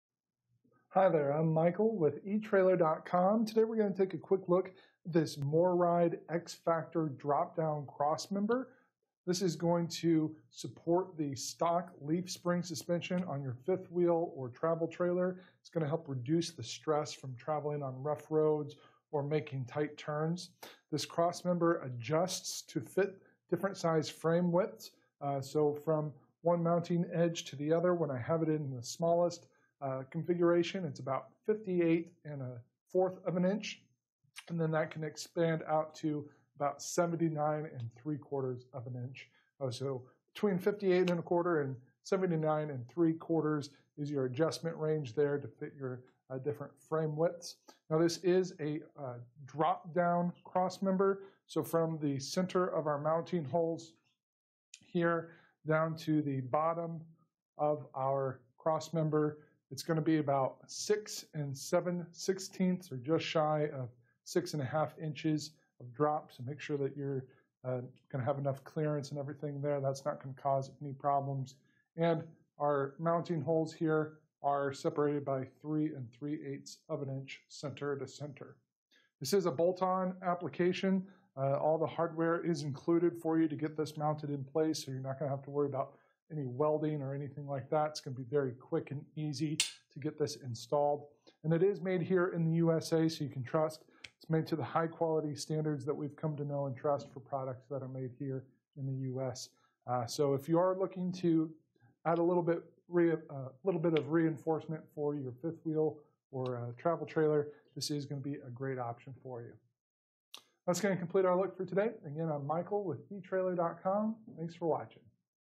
A Closer Look at the MORryde X-Factor Dropdown Crossmember for Stock Leaf Spring Suspension
Click for more info and reviews of this MORryde Trailer Axles,Trailer Suspension Parts:

Hi there, I'm Michael with etrailer.com. Today, we're going to take a quick look at this MORyde X-Factor dropdown crossmember. This is going to support the stock leaf spring suspension on your fifth wheel or travel trailer. It's gonna help reduce the stress from traveling on rough roads or making tight turns. This crossmember adjusts to fit different size frame widths. So from one mounting edge to the other one I have it in the smallest configuration.

It's about 58 1/4 of an inch. And then that can expand out to about 79 3/4 of an inch. So between 58 1/4 and 79 3/4 is your adjustment range there to fit your different frame widths. Now this is a dropdown crossmember. So from the center of our mounting holes here down to the bottom of our crossmember, it's gonna be about 6 7/16 or just shy of 6 1/2 inches of drops so make sure that you're gonna have enough clearance and everything there that's not gonna cause any problems.

And our mounting holes here are separated by 3 3/8 of an inch center to center. This is a bolt-on application. All the hardware is included for you to get this mounted in place so you're not gonna have to worry about any welding or anything like that. It's gonna be very quick and easy to get this installed and it is made here in the USA so you can trust it's made to the high quality standards that we've come to know and trust for products that are made here in the US. So if you are looking to add a little bit of reinforcement for your fifth wheel or travel trailer this is gonna be a great option for you.